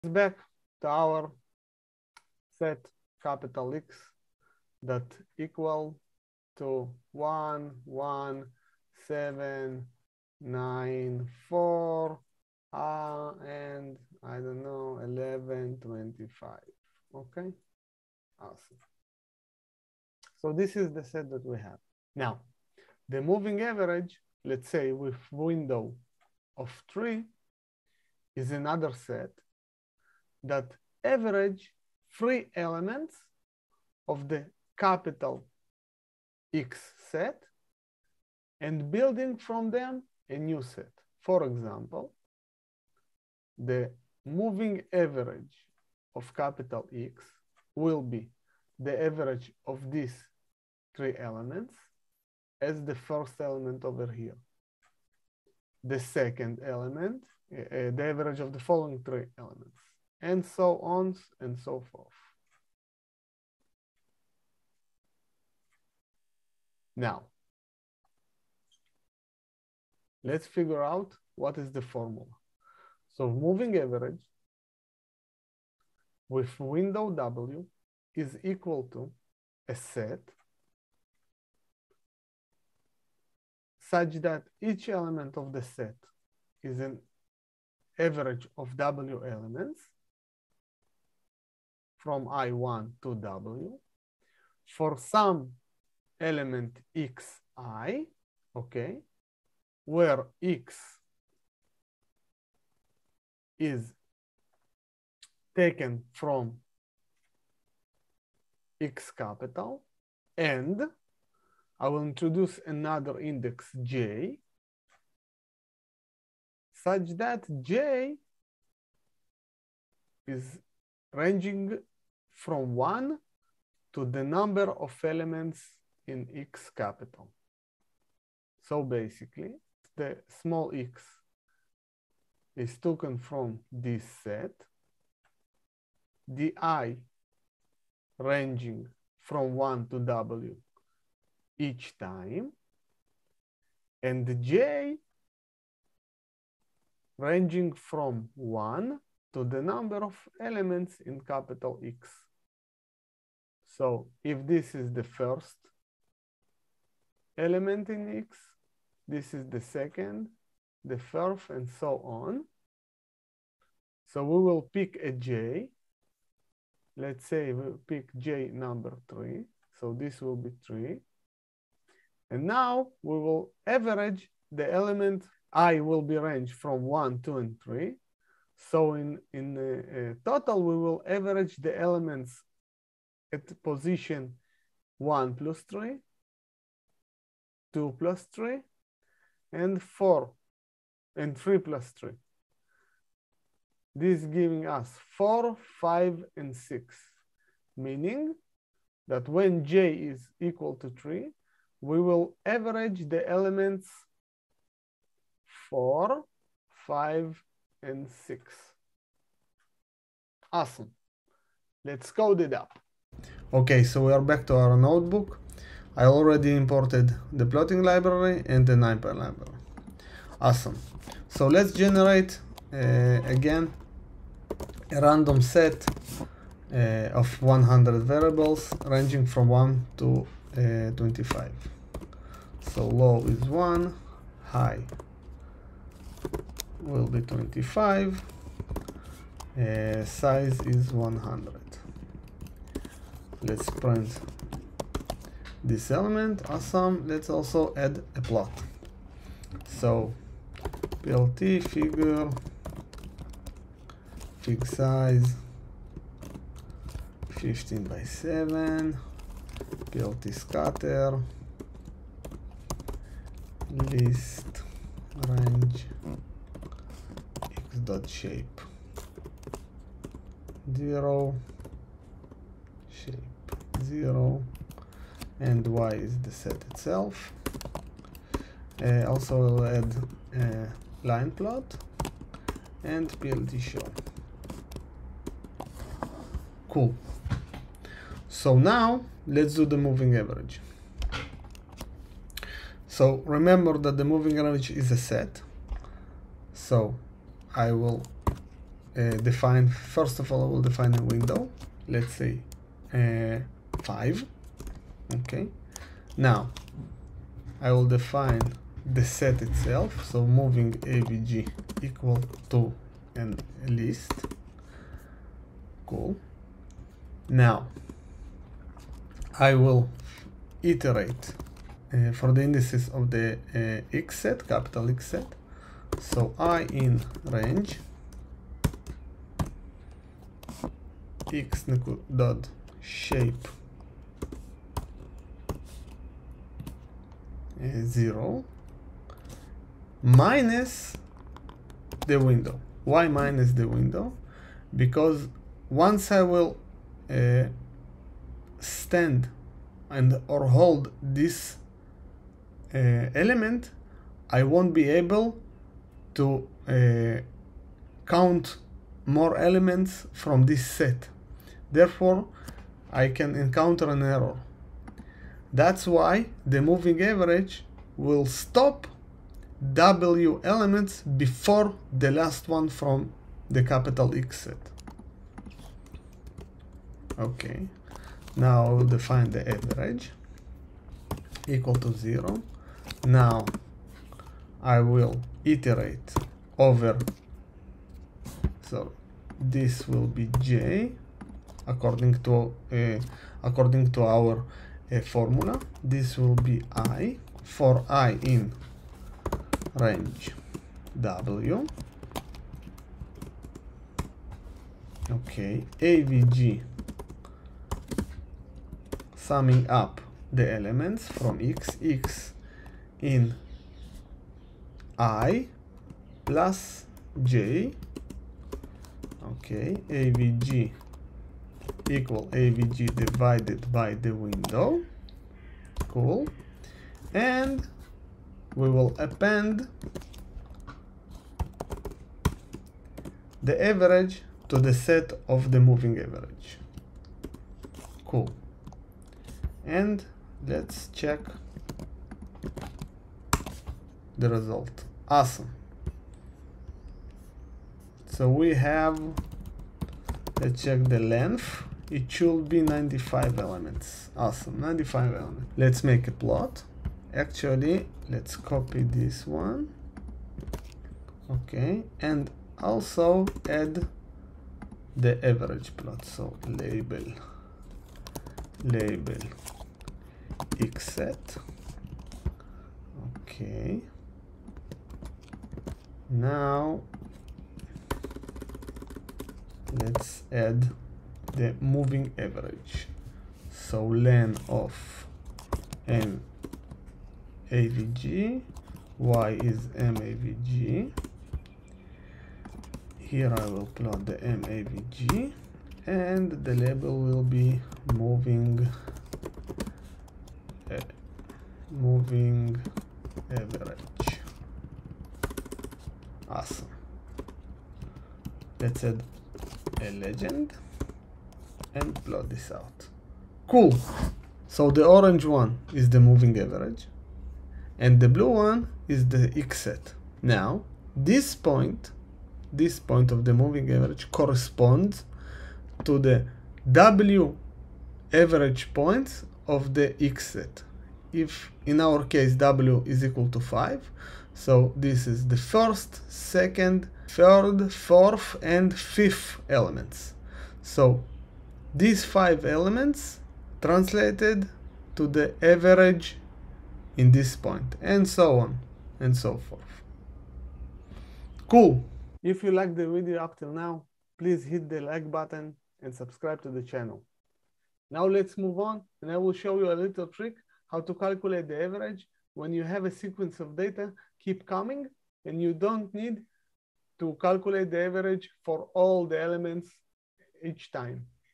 Math Programing - Average, Moving Average, Real time trick Average. Part 2.
In this video I will share with you all you need to know about average, how to calculate it, we will look at the formula and program it using python.